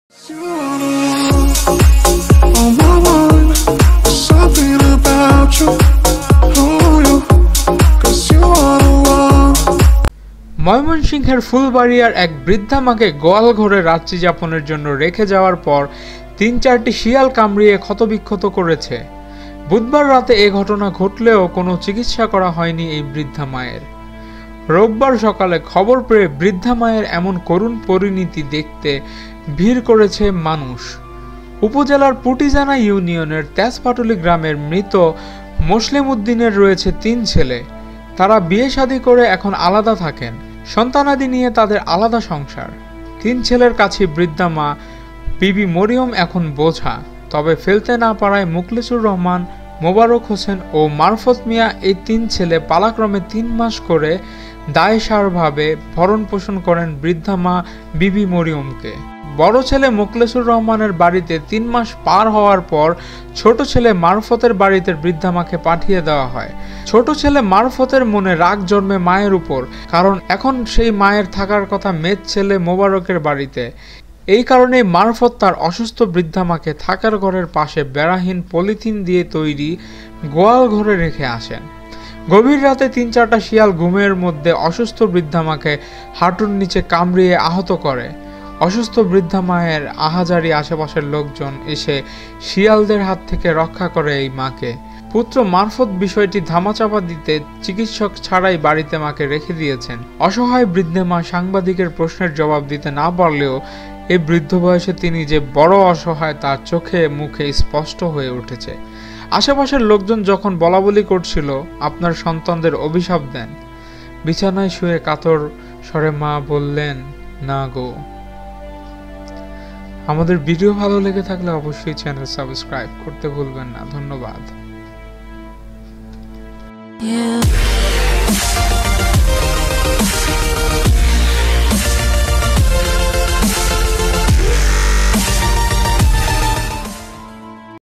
গোয়াল ঘরে গর্ভধারিণী বৃদ্ধা মাকে খাচ্ছে শিয়াল অবাক হবেন দেখলে ! মায়ের স্থান কি গরুর সাথে গোয়াল ঘরে ?
বৃদ্ধা মা বিবি মরিয়ম এখন বোঝা স্থান গোয়াল ঘরে

ময়মনসিংহের ফুলবাড়ীয়ায় এক বৃদ্ধা মাকে গোয়াল ঘরে রাত্রিযাপনের জন্য রেখে যাওয়ার পর ৩/৪টি শিয়াল কামড়িয়ে ক্ষত বিক্ষত করেছে। বুধবার রাতে এ ঘটনা ঘটলেও কোনো চিকিৎসা করানো হয়নি বৃদ্ধা মায়ের।

রোববার সকালে খবর পেয়ে বৃদ্ধা মায়ের এমন করুণ পরিণতি দেখতে ভিড় করছে মানুষ।

উপজেলার পুটিজানা ইউনিয়নের তেজপাটুলী গ্রামের মৃত মোসলেম উদ্দিনের রয়েছে তিন ছেলে। তারা বিয়েশাদি করে এখন আলাদা। সন্তান-সন্তানাদি নিয়ে তাদের আলাদা সংসার।

তিন ছেলের কাছেই বৃদ্ধা মা বিবি মরিয়ম (৮৫) এখন বোঝা। তবে ফেলতে না পারায় মোখলেছুর রহমান, মোবারক হোসেন, মারফত মিয়া- এই তিন ছেলে পালাক্রমে তিন মাস করে দায়সাড়াভাবে ভরনপোষণ করেন বৃদ্ধা মা বিবি মরিয়মকে।

গর্ভধারিণী এক মায়ের স্থান হয়েছে ভাঙ্গা গোয়াল ঘরে গরুর সঙ্গে।